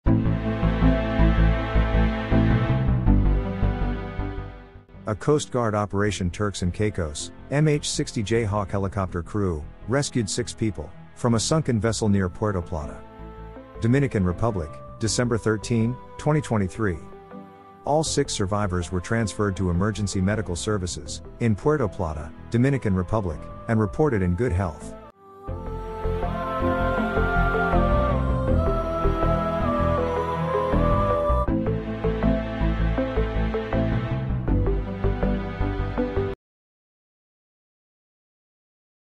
Coast Guard rescues 6 people from sunken vessel off Dominican Republic
A Coast Guard Operation Turks and Caicos, MH-60 Jayhawk helicopter crew rescued, six people from a sunken vessel near Puerto Plata, Dominican Republic, Dec. 13, 2023.

PUERTO PLATA, DOMINICAN REPUBLIC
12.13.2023

All six survivors were transferred to emergency medical services in Puerto Plata, Dominican Republic and reported in good health. (U.S. Coast Guard video)